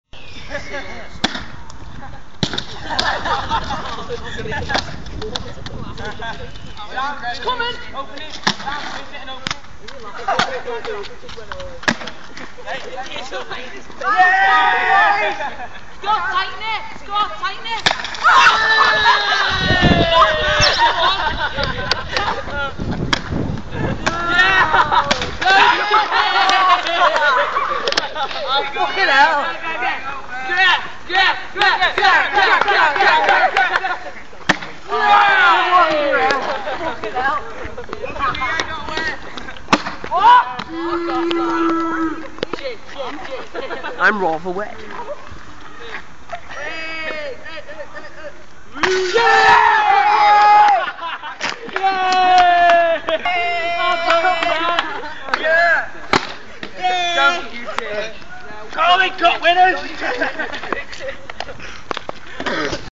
coke bomb
coke exploding without mentos